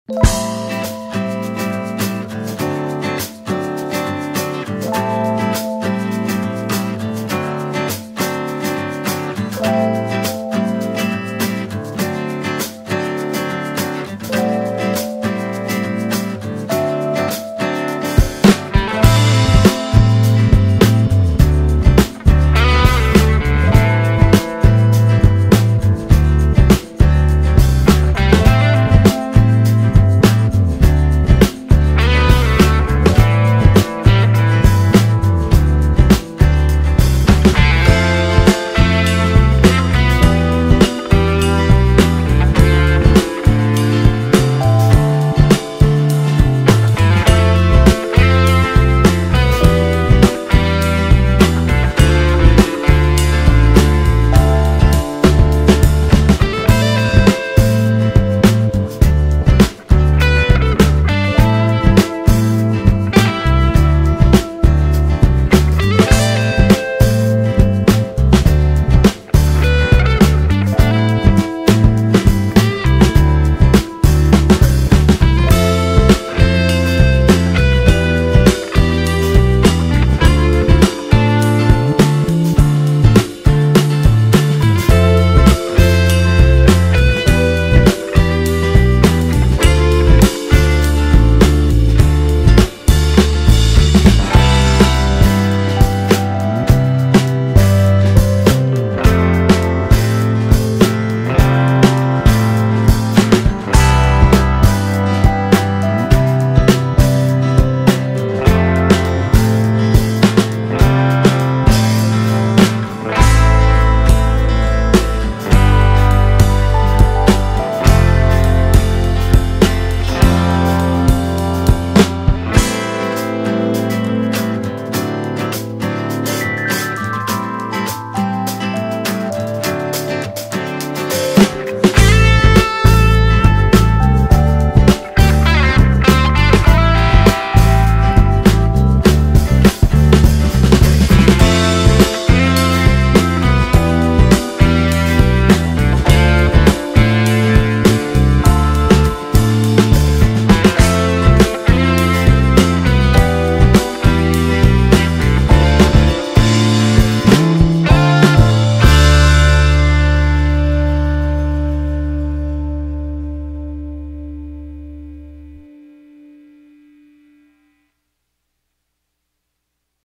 I transformed Naruto characters into Live Action (Team Kakashi)
I took some screencaps of Naruto characters and generated live action versions by using AI to trace image to image, as well as animated video.